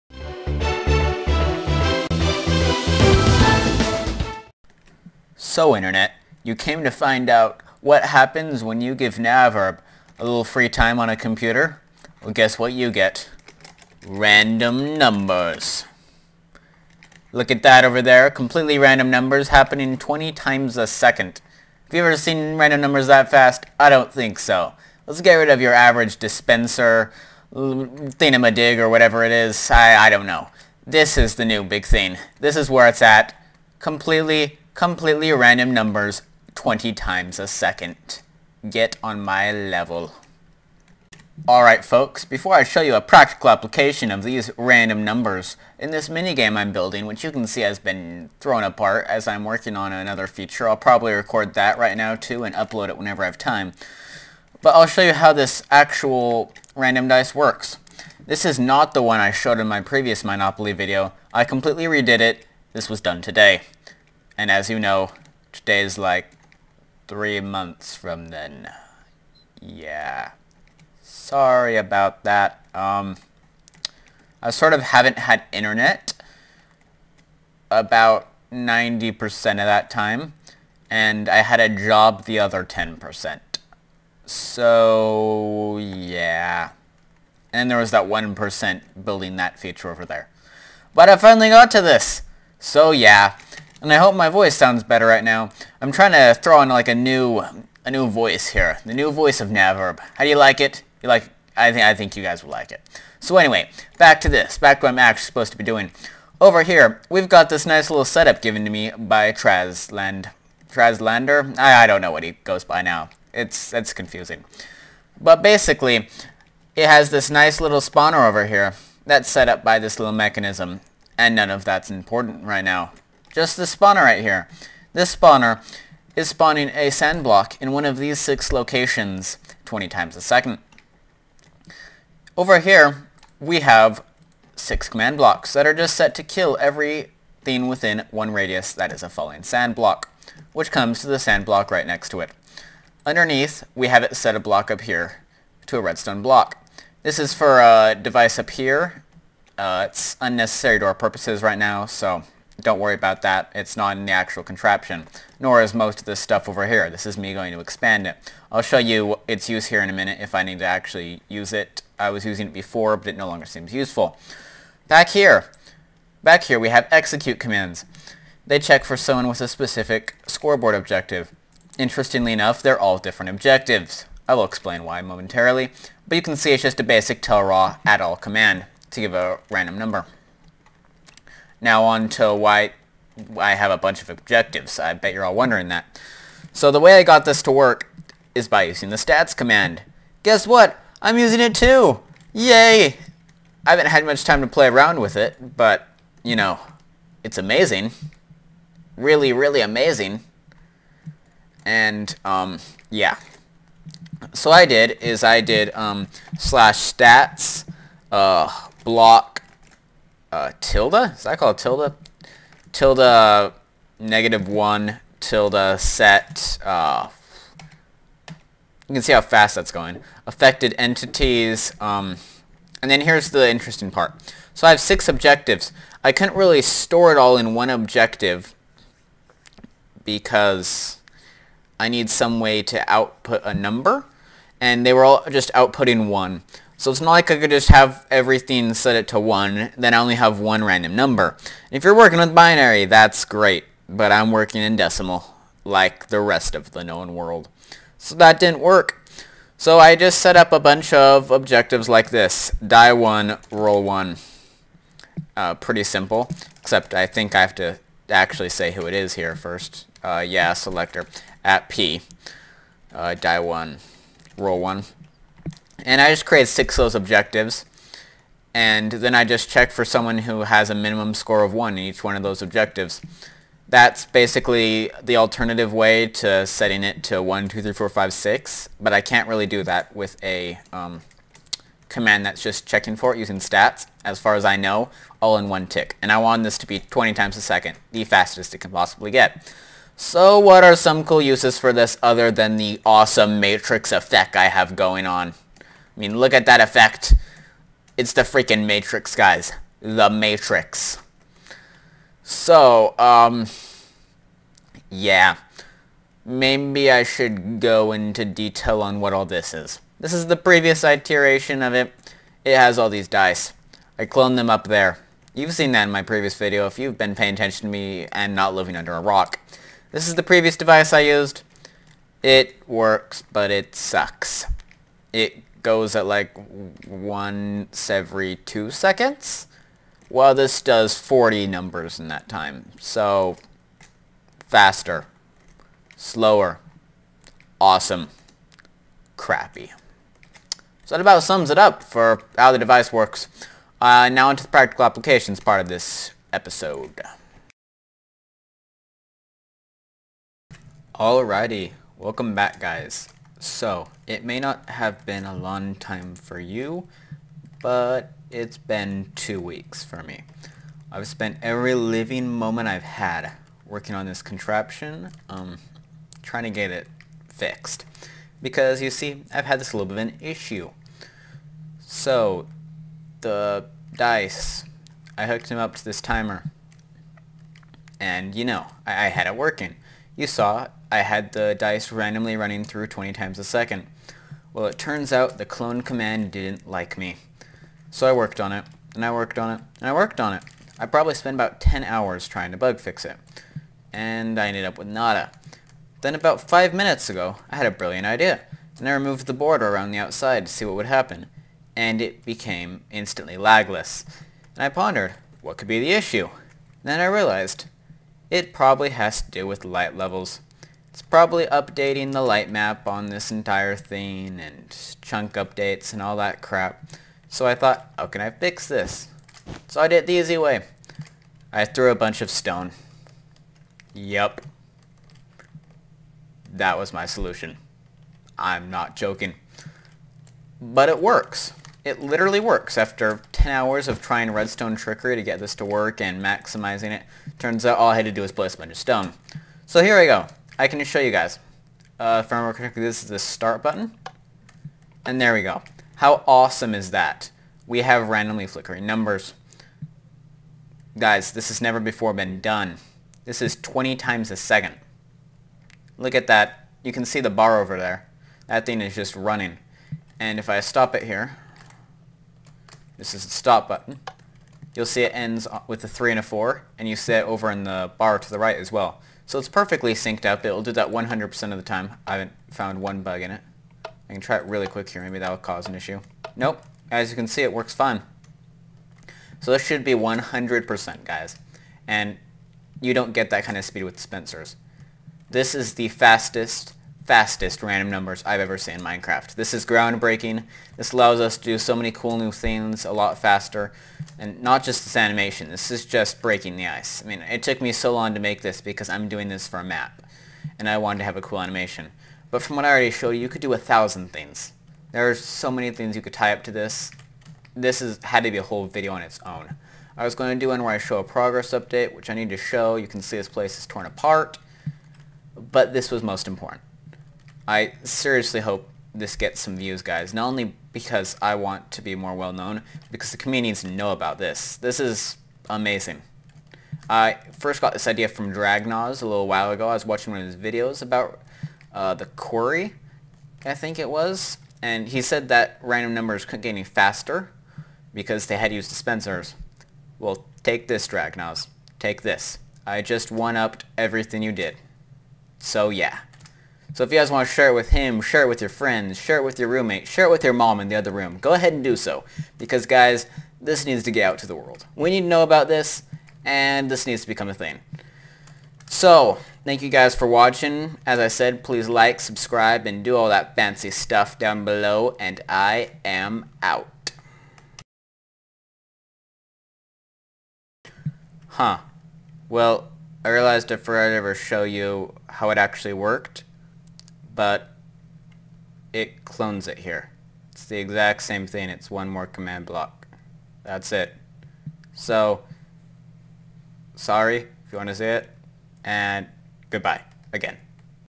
Ultra-Fast Random Numbers (20x Per Second!)
Take that Dragnoz. Take that.  ;)

(One of my commentators recommended spreadplayers; you can read why that will not work below)

"spreadplayers only allows for the squares of odd numbers, namely 1², 3², 5²... (the formula would thus be (2n-1)²)

In my case, the spawners allow for any number to be used. Also, spreadplayers causes fallingSand to land, which means you would have to constantly be clearing the area of sand and then summoning more, which would slow down the device a lot while it waits for the sand to land, or use a mob, which requires extra headache to add a bunch of tags to the mob.

To seal the deal, spreadplayers doesn't move mobs around at 20X per second. It actually moves mobs over an area in a line over multiple ticks, meaning that in a test I did with 9 random numbers at least 5 were active at any given time, and the mob was only moving to a new block at about 3-4 times per second.

So in all, no. Spreadplayers would not work for 20X per second speed. Does that clear the matter up a bit?"